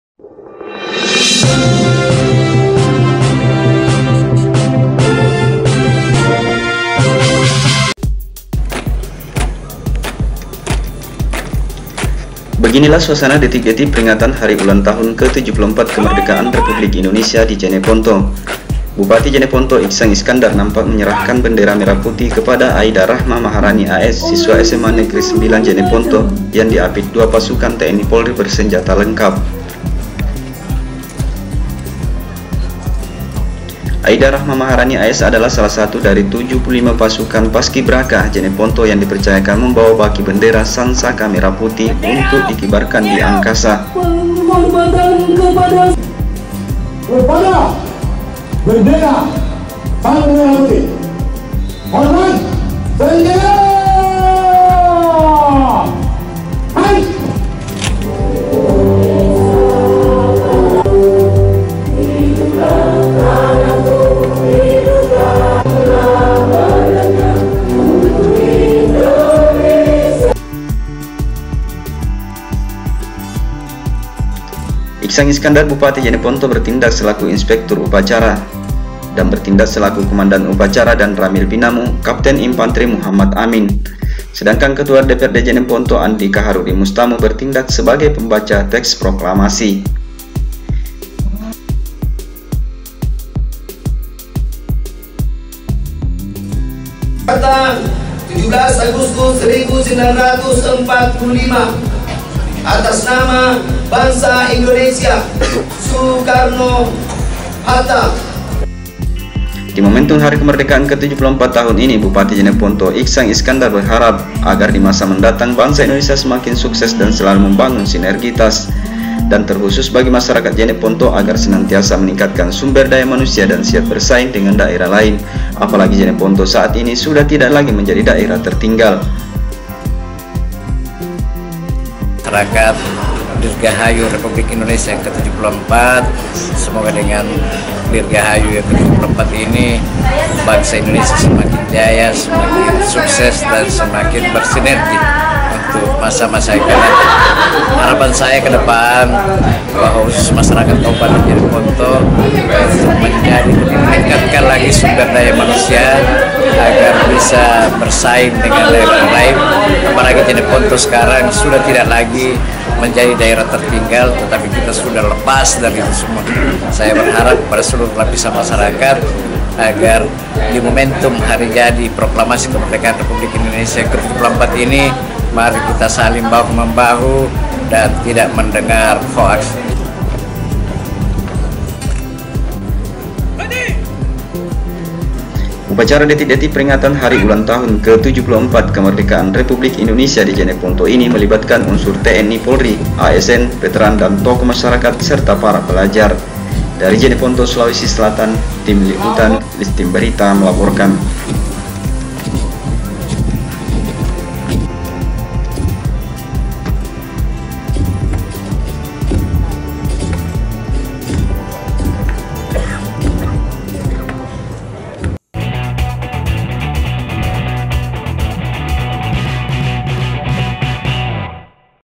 Detik Detik Pengibaran Sang Saka Merah Putih di Jeneponto
Beginilah suasana Detik-detik peringatan Hari Ulang Tahun ke 74 kemerdekaan Republik Indonesia di Jeneponto. Bupati Jeneponto Iksan Iskandar nampak menyerahkan bendera merah putih kepada Aidah Rahma Maharani AS siswi SMA Negeri 9 Jeneponto yang diapit dua pasukan TNI/Polri bersenjata lengkap.

Aidah Rahma Maharani AS  adalah salah satu dari 75 pasu kan paskibraka Jeneponto yang dipercayakan membawa baki bendera sang saka merah putih untuk dikibarkan di angkasa.